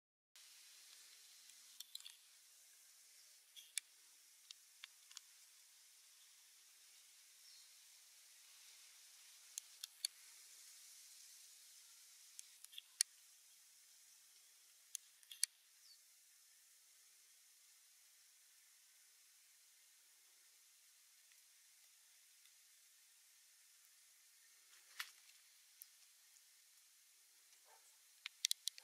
Dominican Ameiva (Pholidoscelis fuscatus), endemic for the island species
Filmed on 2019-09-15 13:45h near Mero, Dominica (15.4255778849, -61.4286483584) with Panasonic TZ80 (stabilized)